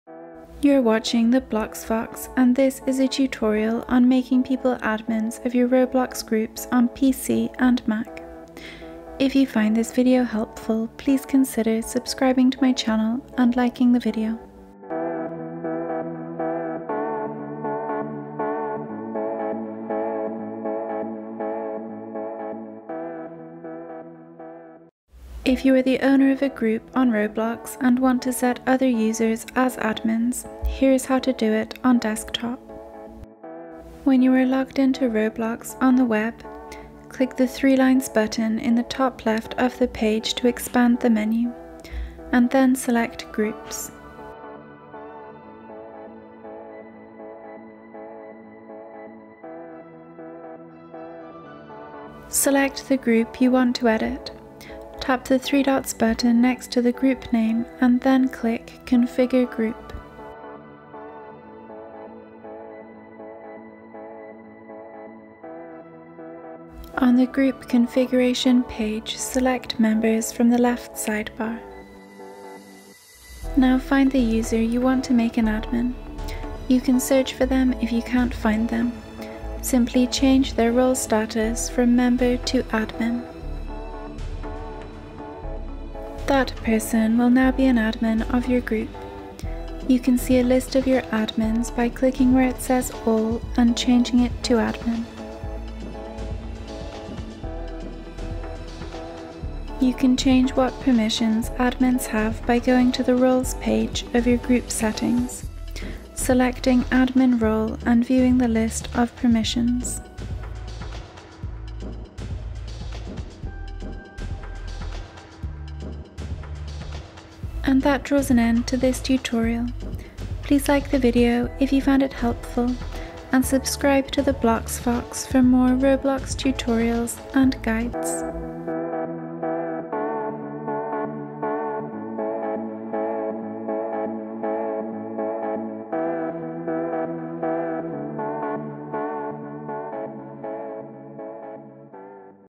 How To Make Someone Admin Of Roblox Group (PC & Mac)
Tutorial on setting group users as admins on Roblox Groups for the desktop version of Roblox on PC & Mac. If you are the owner of a Roblox group, you can let other people become admins. Learn how to make someone the admin of a Roblox group on desktop now!

Steps:

If you are the owner of a group on Roblox and want to set other users as admins here is how to do it on Desktop.

When you are logged in to Roblox on the web, click the ‘3 lines button’ on the top left of the page to expand the menu, and then select ‘groups’.

Select the group you want to edit, Tap the 3 dots button next to the group name and then click ‘configure group’.

On the group configuration page, select ‘members’ from the left sidebar.

Now find the user you want to make an admin, you can search for them if you can’t find them. Simply change their role status from ‘Member’ to ‘Admin’.

That person will now be an admin of your group. You can see a list of your admins by clicking where it says ‘all’ and changing it to ‘admin’.

You can change what permissions Admins have by going to the ‘roles’ page of your group settings, selecting ‘admin’ role and viewing the list of permissions.